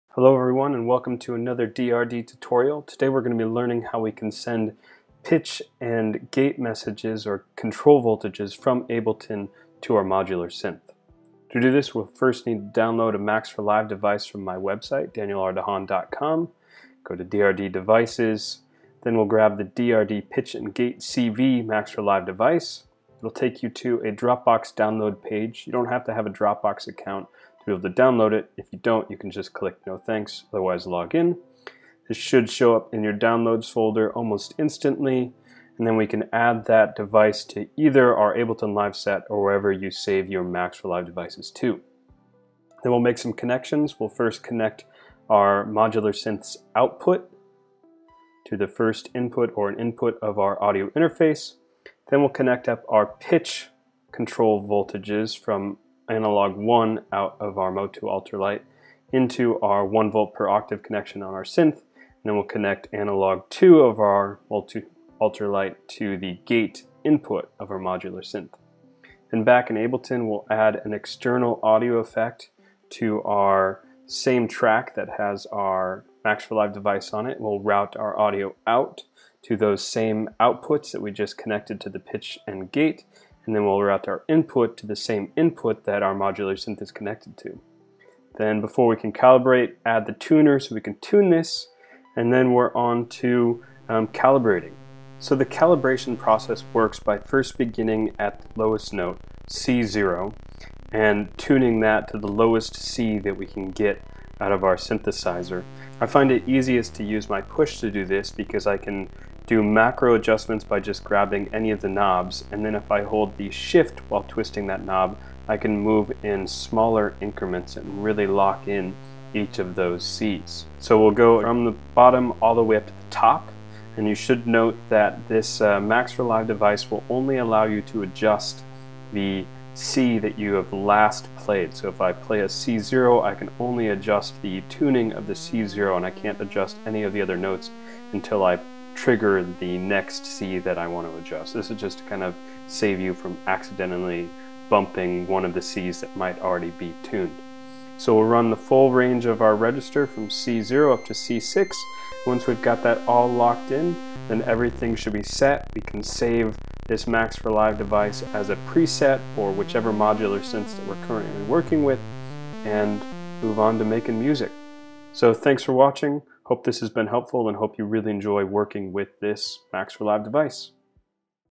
Controlling You Modular Synth from Ableton Live
This video shows you how to download, install, and setup the DRD_Pitch&Gate-CV~, a Max for Live Device that enables you to translate MIDI note and velocity messages to 1v/oct Pitch and 5v Gate control voltages for controlling you modular synth.

Required Materials:
 - A DC-Coupled Audio Interface (i.e. the Motu Ultralight Mk3, or similar)
 - DRD_Pitch&Gate-CV~